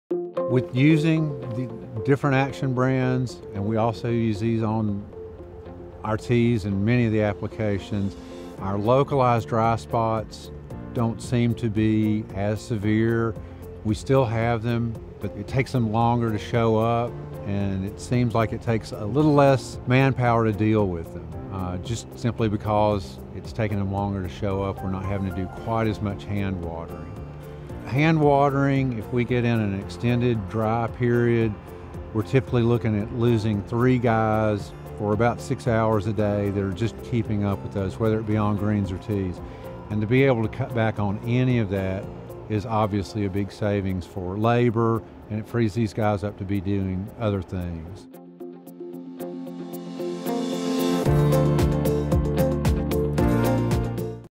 Saving Time with Action Brand Fungicides
Josh Smith, golf course superintendent at Elk River Club in Banner Elk, North Carolina gives insight on how utilizing the different Action™ brand fungicides has saved his team time and labor by being able to cut back on hand watering.

Key Moment Timestamps:

00:00:01 – Action Brands saving time

00:00:28 – Hand watering and Action Brands

00:00:49 – Learn More